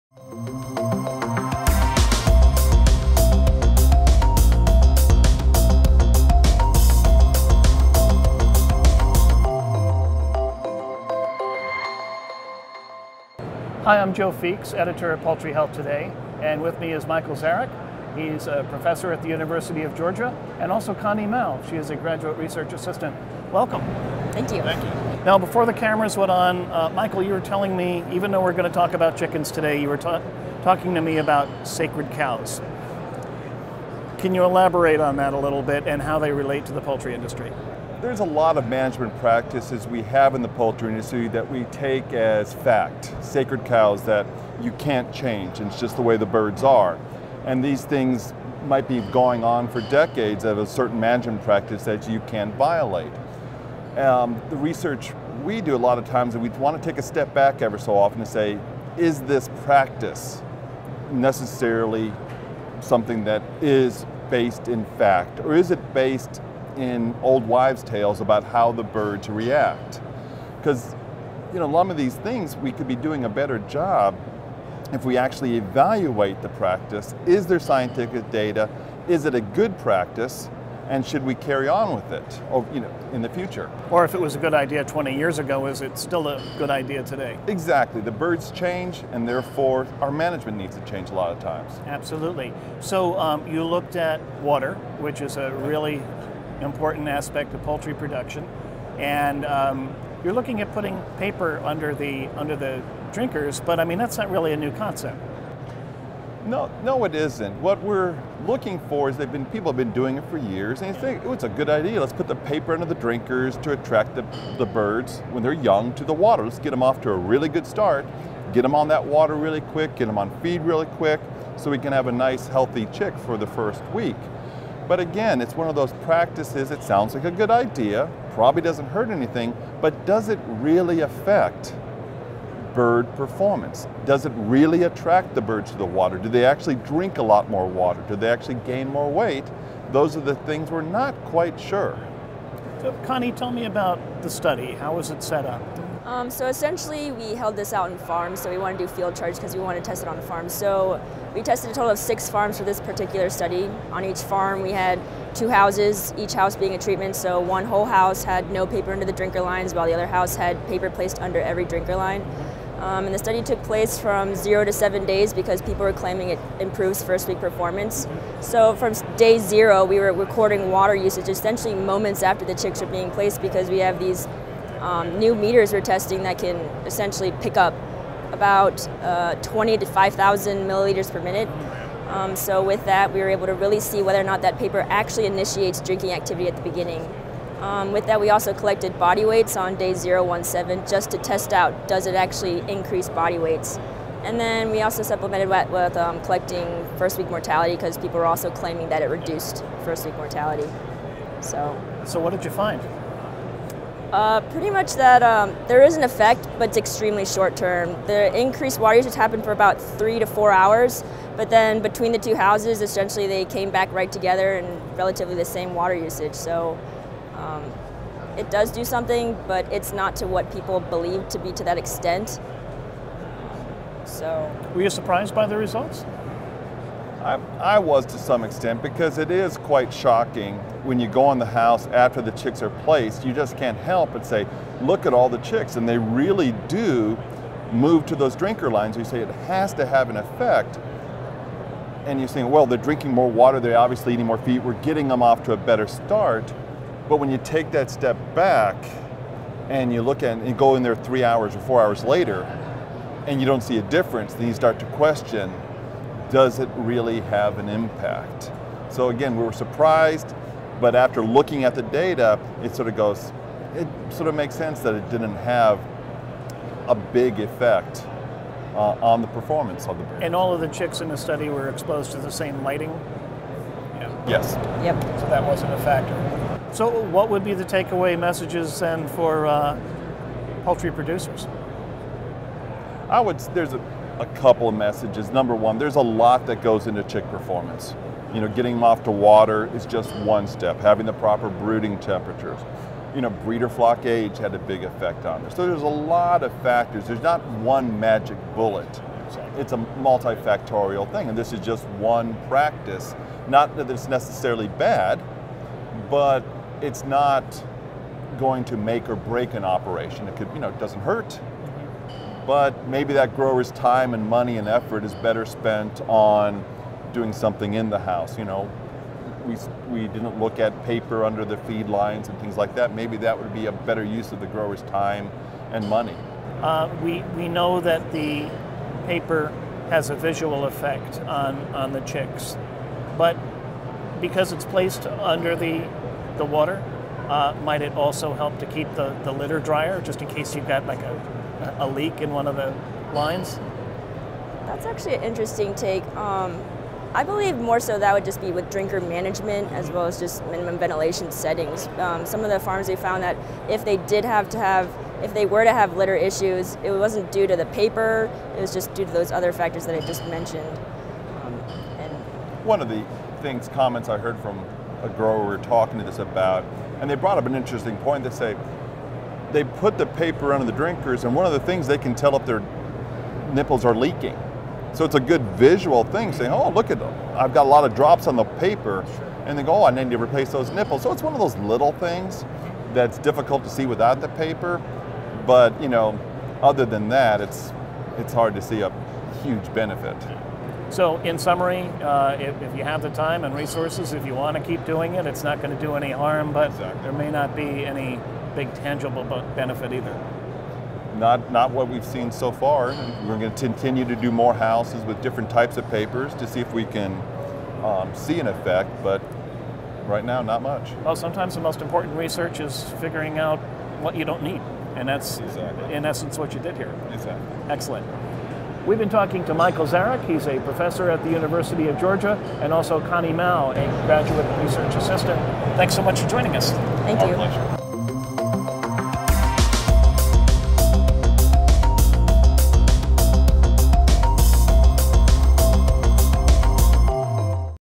Putting paper under chick’s drinkers: Is it really helpful?
Placing paper under water lines increases water consumption by baby chicks, but only for the first 3 or 4 hours after house placement, research indicates.
When day-old chicks placed in a house readily move to drinker lines with paper underneath, it’s thought that paper is the attraction, an assumption tested in a study by Michael Czarick, a professor at the University of Georgia, and graduate research assistant Connie Mou.